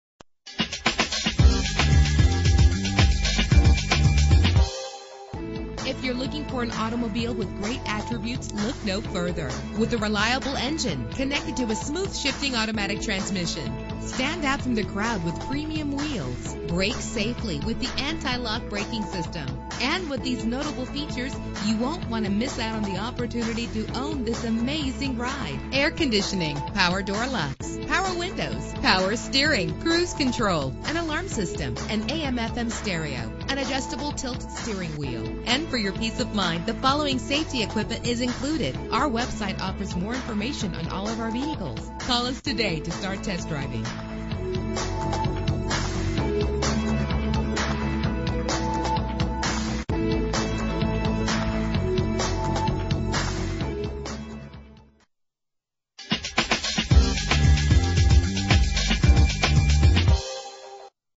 2002 Ford Windstar Passenger - Manheim PA
Year: 2002
Make: Ford
Model: Windstar Passenger
Trim: LX Minivan
Engine: 6 cylinder 3.8 liter
Transmission:
Color: Green
Mileage: 68841
Address:
14 Anthony Drive